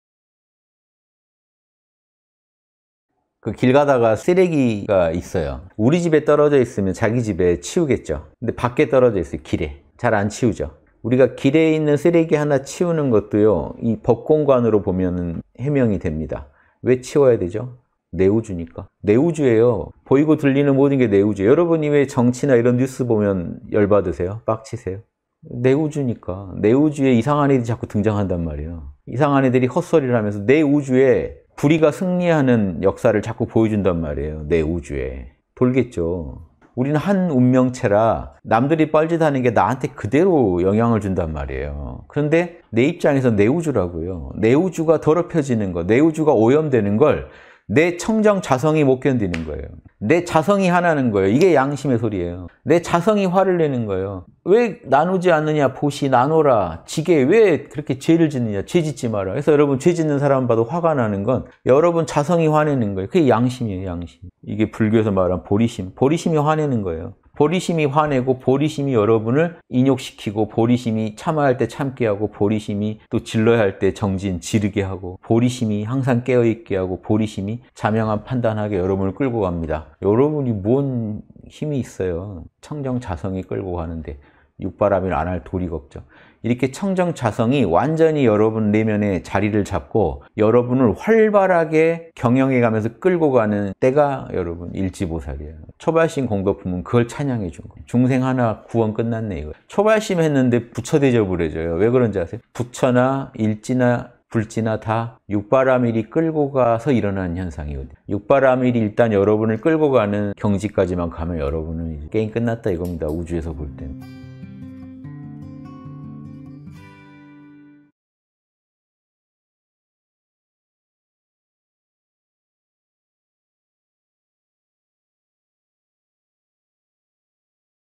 [3분 인문학] 내 우주의 쓰레기는 내가 치운다
230401 정토라이브

길 가다가 쓰레기가 있어요.
우리 집에 떨어져 있으면
자기 집에, 치우겠죠.
근데 밖에 떨어져 있어요, 길에.
잘 안 치우죠.
우리가 길에 있는 쓰레기 하나
치우는 것도요,
이 법공관으로 보면
해명이 됩니다.
왜 치워야 하죠?
내 우주니까.
내 우주예요.
보이고 들리는 모든 게
내 우주예요.
여러분이 왜 정치나 이런 뉴스 보면
열 받으세요? 빡 치세요?
내 우주니까.
내 우주에 이상한 애들이
자꾸 등장한단 말이에요.

이상한 애들이 헛소리를 하면서
내 우주에 불의가 승리하는
역사를 자꾸 보여준단 말이에요,
내 우주에. 돌겠죠.
우리는 한 운명체라
남들이 뻘짓 하는 게
나한테 그대로
영향을 준단 말이에요.
그런데 내 입장에선
내 우주라고요.
내 우주가 더럽혀지는 거,
내 우주가 오염되는 걸
내 청정 자성이 못 견디는 거예요.
내 자성이 화나는 거예요.
이게 양심의 소리예요.
내 자성이 화를 내는 거예요.
왜 나누지 않느냐?
보시, 나눠라.
지계, 왜 그렇게 죄를 짓느냐?
죄 짓지 마라.

그래서 여러분 죄 짓는 사람 봐도
화가 나는 건
여러분 자성이 화내는 거예요.
그게 양심이에요, 양심.
이게 불교에서 말한 보리심,
보리심이 화내는 거예요.
보리심이 화내고
보리심이 여러분을 인욕시키고,
보리심이 참아야 할 때
참게 하고 보리심이
또 질러야 할 때
정진, 지르게 하고
보리심이 항상 깨어 있게 하고
보리심이 자명한 판단 하게
여러분을 끌고 갑니다.
여러분이 뭔 힘이 있어요.
청정 자성이 끌고 가는데
6바라밀 안 할 도리가 없죠.

이렇게 청정 자성이 완전히
여러분 내면에 자리를 잡고
여러분을 활발하게 경영해 가면서
끌고 가는 때가 여러분,
1지 보살이에요.
초발심 공덕품은 그걸
찬양해 준 거예요.
중생 하나 구원 끝났네,
이거에요.
초발심 했는데 부처 대접을 해줘요.
왜 그런지 아세요?
부처나 1지나 불지나 다
6바라밀이 끌고 가서
일어나는 현상이거든요.
6바라밀이 일단 여러분을 끌고 가는
경지까지 가면
여러분은 게임 끝났다 이겁니다.
우주에서 볼 때는.